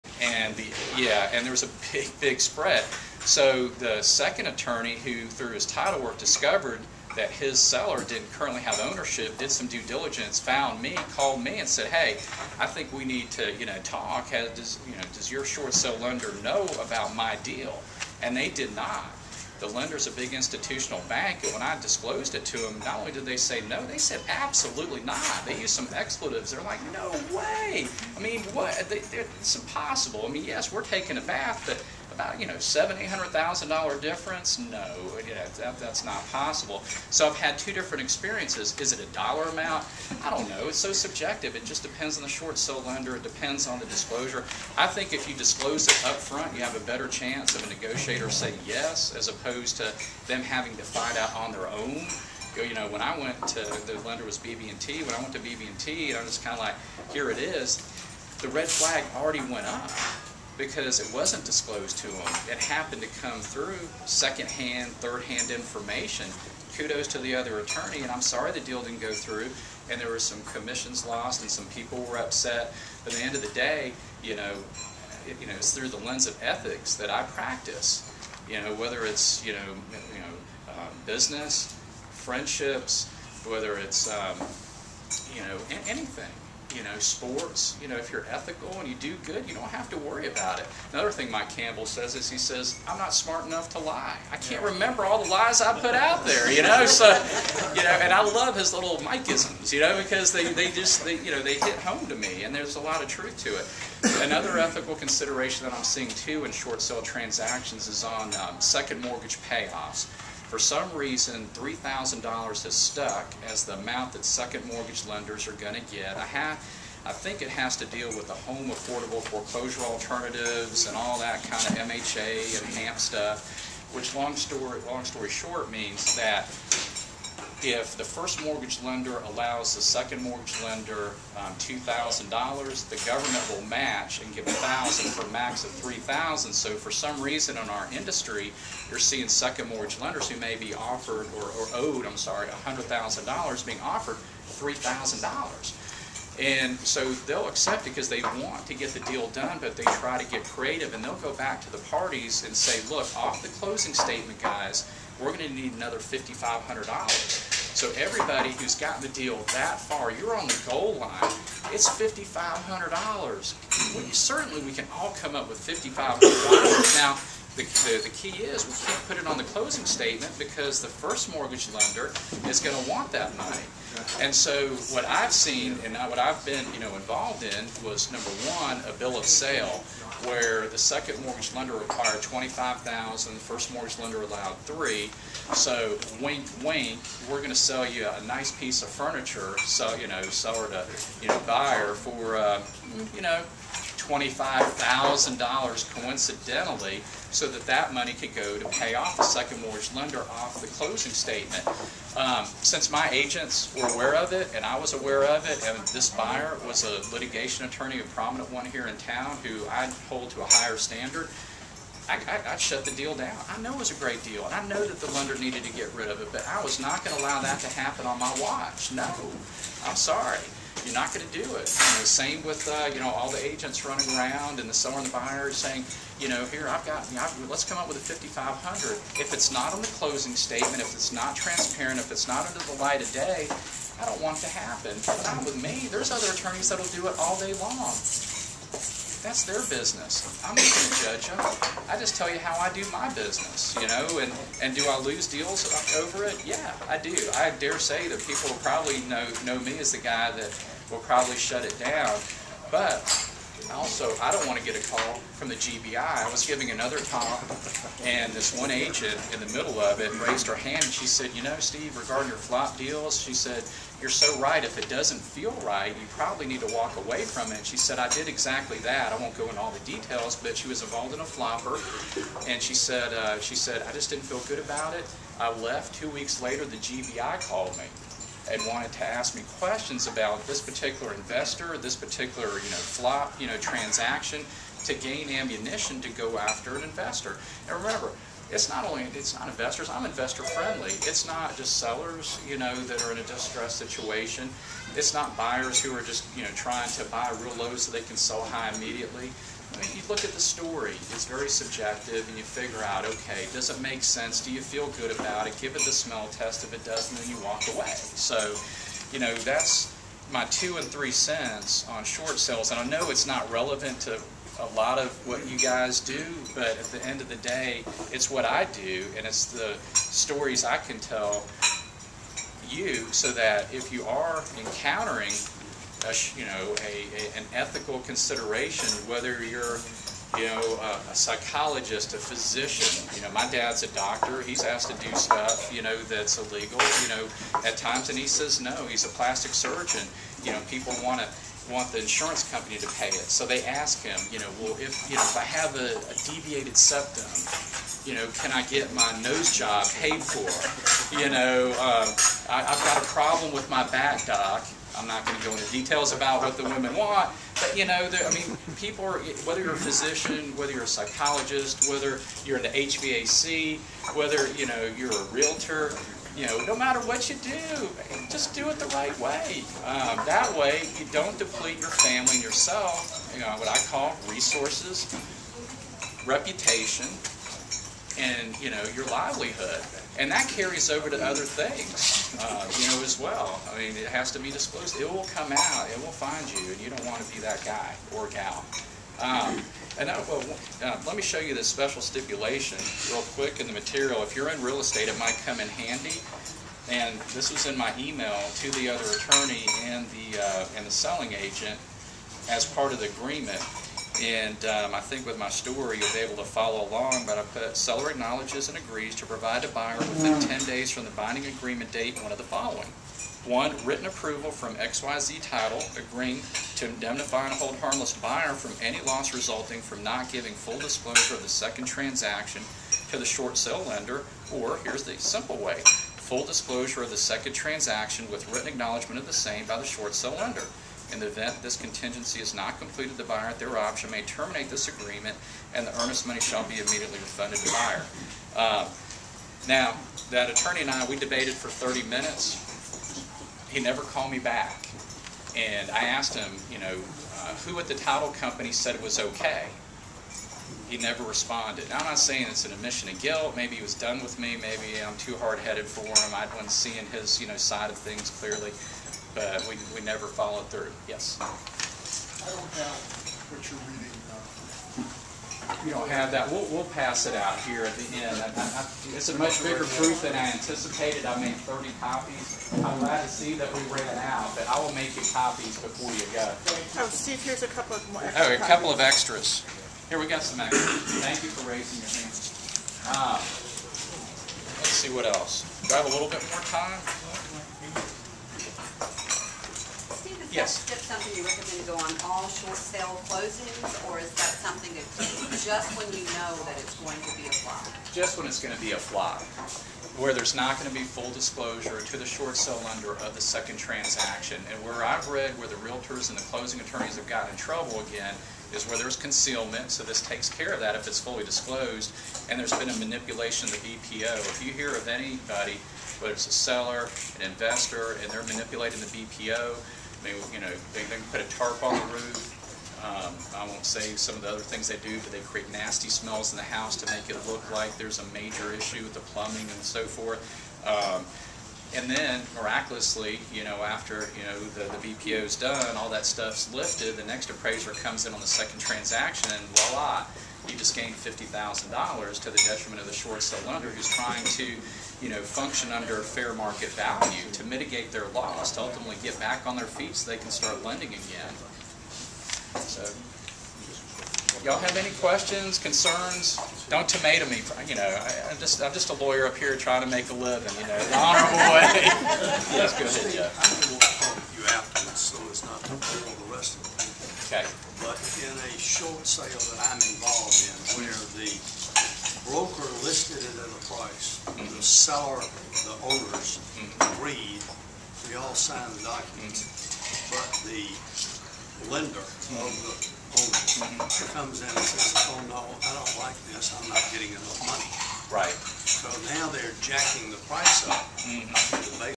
Dagmar Sands has a guest - Steve Golden at Meetup in Johns Creek, GA
- Dagmar Sands with Real Estate International and Johns Creek City Guide invites attorney Steve Golden to be our speaker at the Johns Creek Meetup